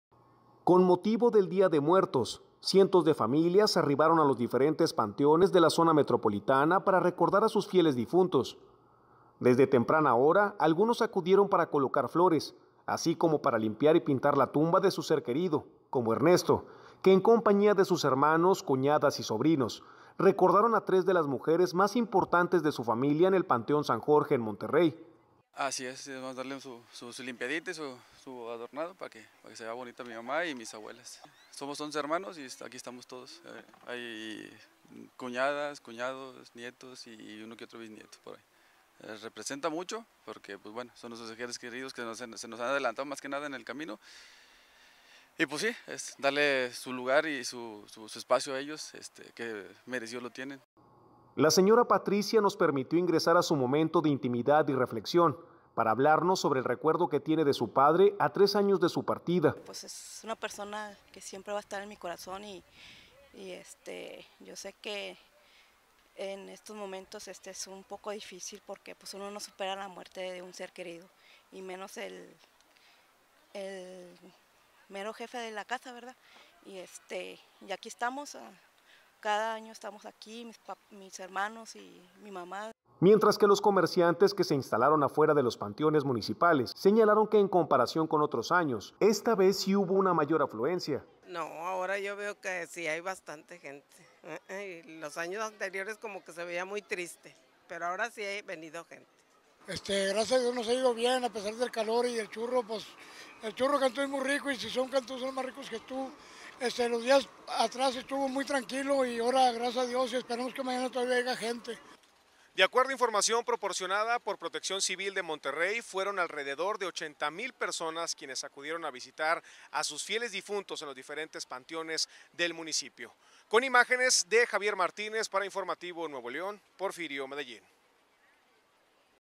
80 mil personas visitan panteones en Día de Muertos
Alrededor de 80 mil personas visitaron a sus fieles difuntos en los diferentes panteones de Monterrey, informó Protección Civil municipal.